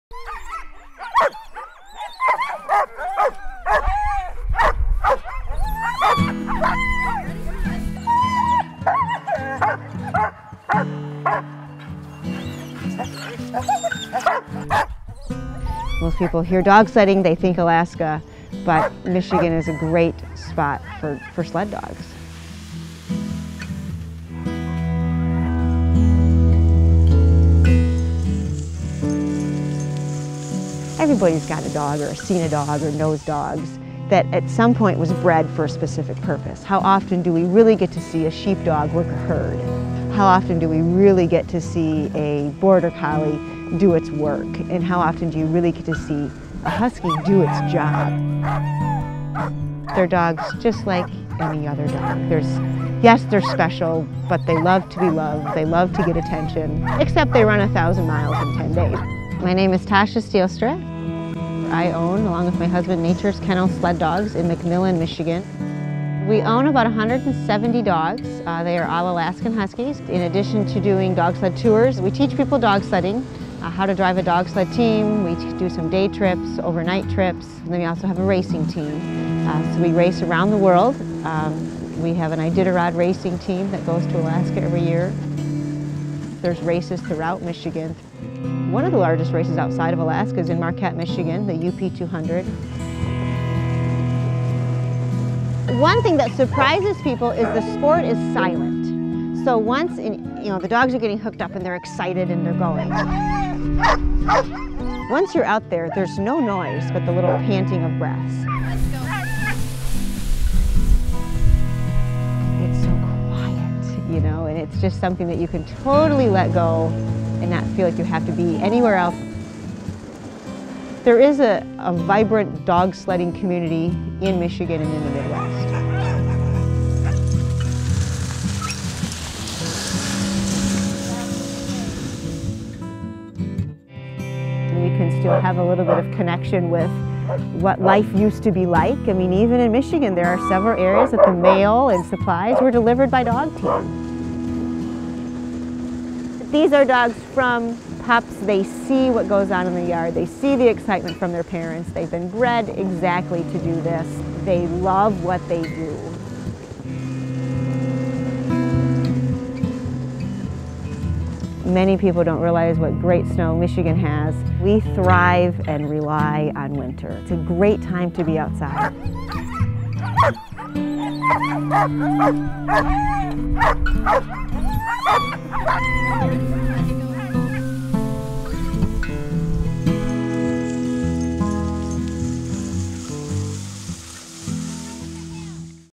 RnR Adventures Presents Dogsledding
Looking for the perfect present for the dog-lover in your life? We're accepting bookings for a 5-day dogsledding adventure in Pure Michigan.